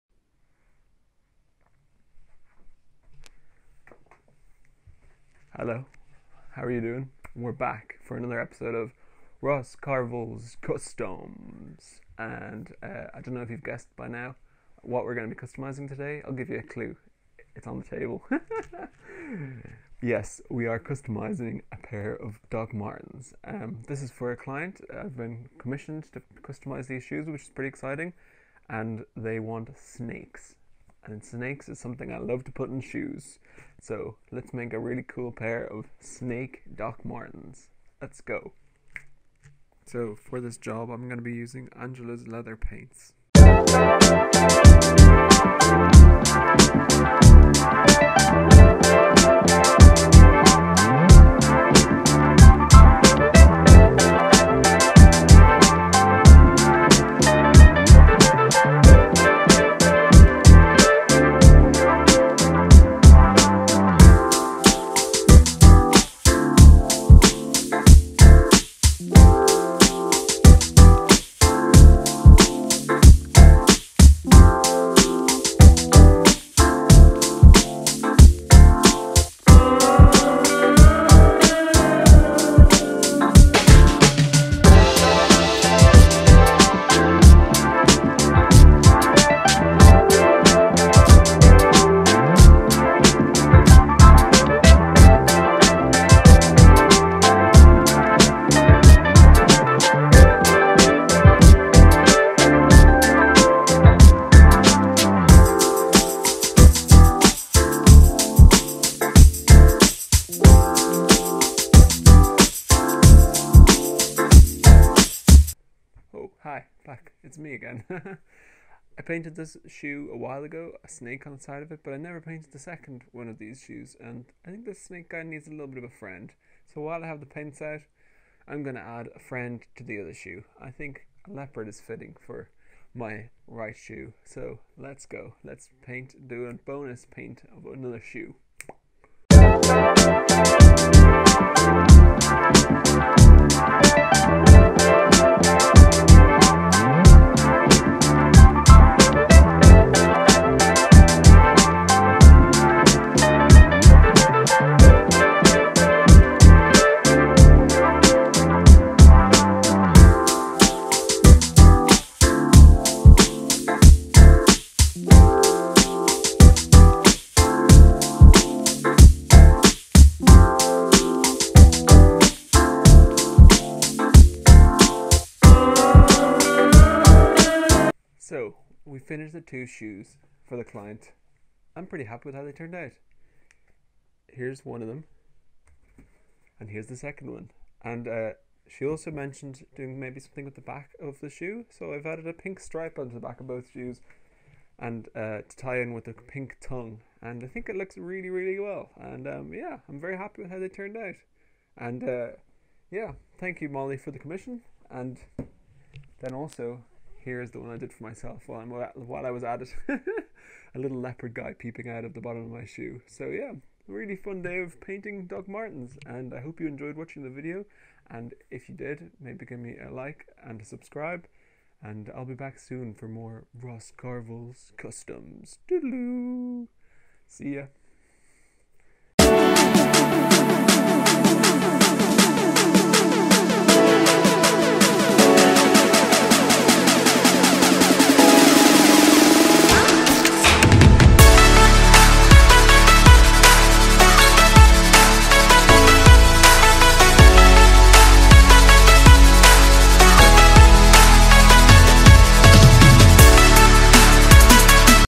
CUSTOM DR. MARTENS!
The latest Ross Carvill Customs! I was commissioned to paint snakes on a pair of Dr.Martens shoes. Thank you Molly for the commission! There is also a bonus customisation at the end of the video! I hope you enjoy watching the process!

If you would like to see more of my work my Instagram is
ross_carvill